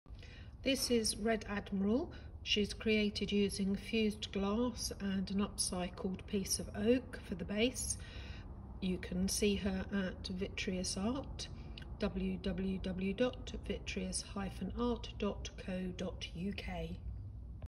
Red Admiral - Fused Glass Art by Jenny Timms at Vitreus Art - £95
A unique piece of fused glass art, made by Jenny Timms, from Vitreus Art.
Mounted on a plinth made from reclaimed Oak, finished in Danish wood oil, the glass butterfly floats gently on a metal support.